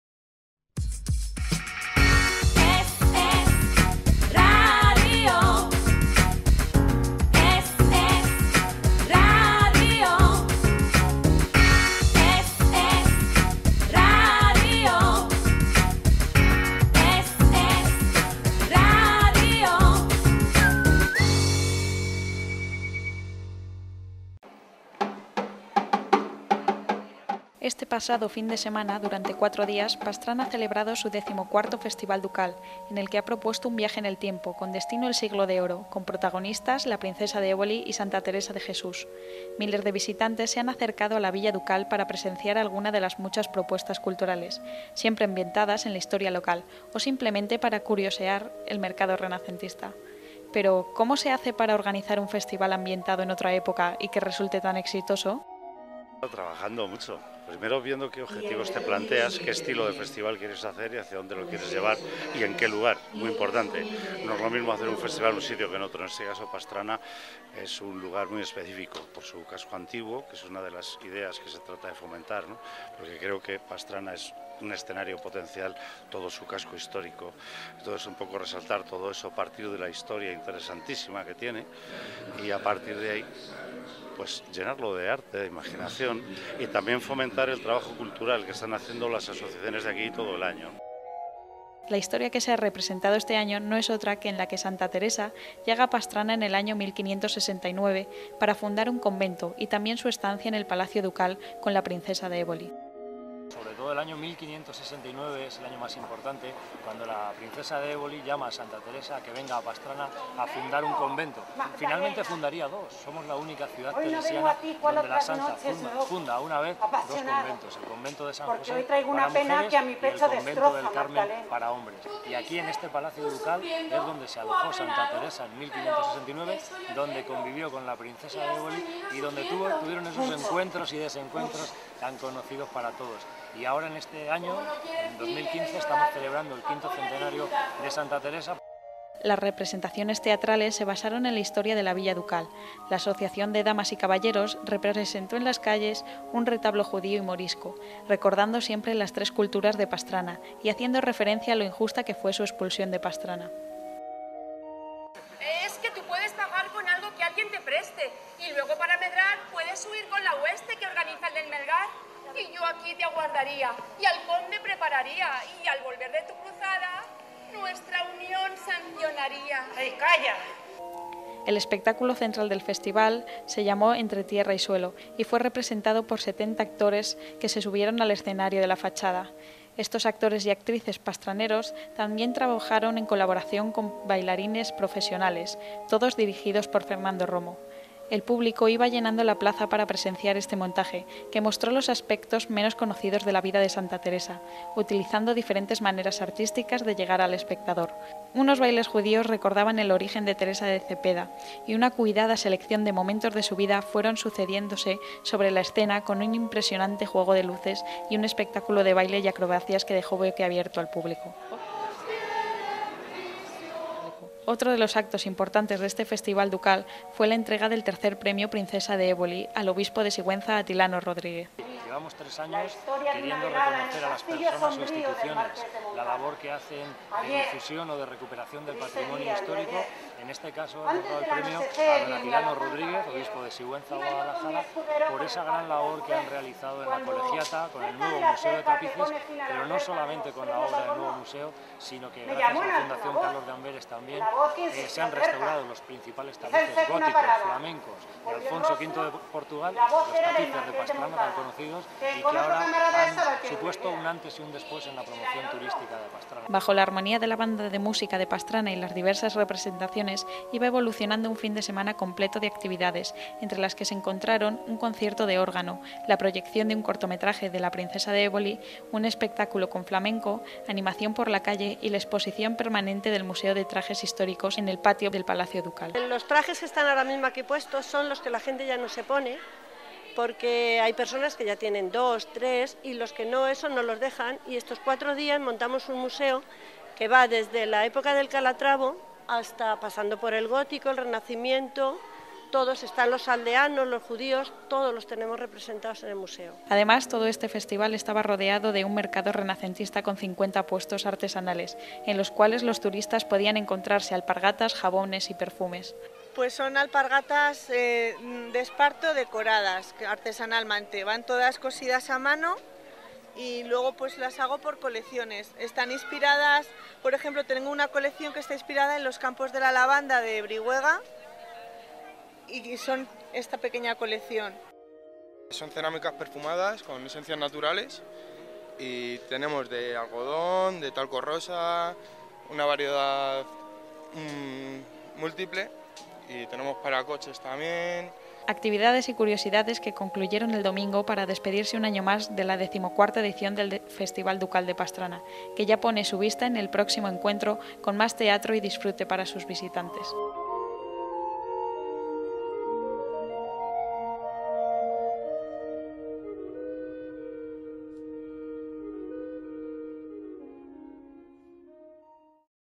Festival ducal de Pastrana
El Festival ducal de Pastrana recrea año tras año la historia de esta villa a través de algunos de los personajes más ilustres que por ella han pasado.